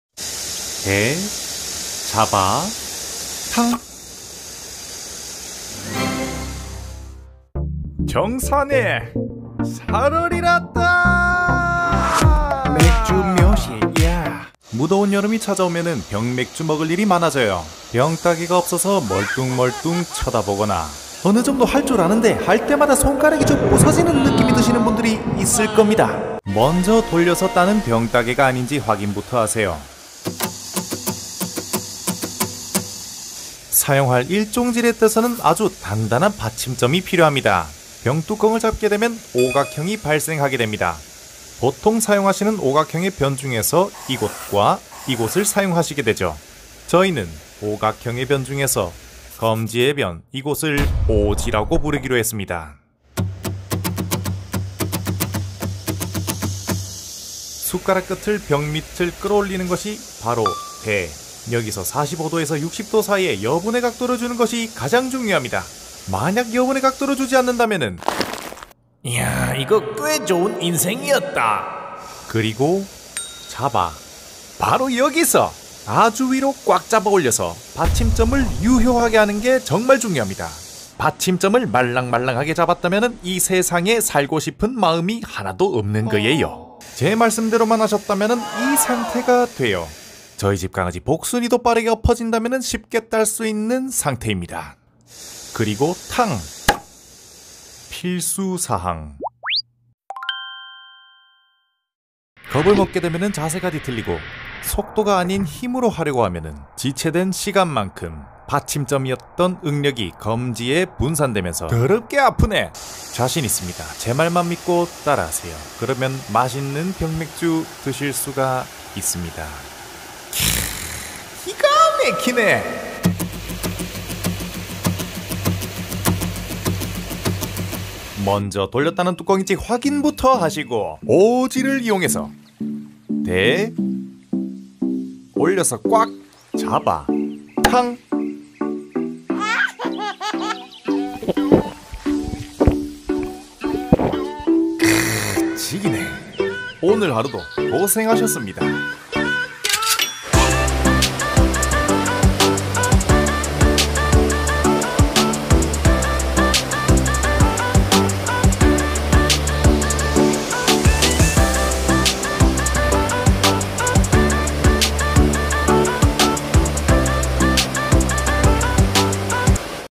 이 '공식' 하나로 종결합니다 - 묘선생의 숟가락으로 병따기
숟가락으로 나도 따고 싶다!
기억하세요.
오지와 대자바탕

묘시네 놀러가보까?

00:00 인트로
00:30 오지
00:54 대자바탕
02:50 따서 무보기

모든 것은 개인 사비로 이루어집니다.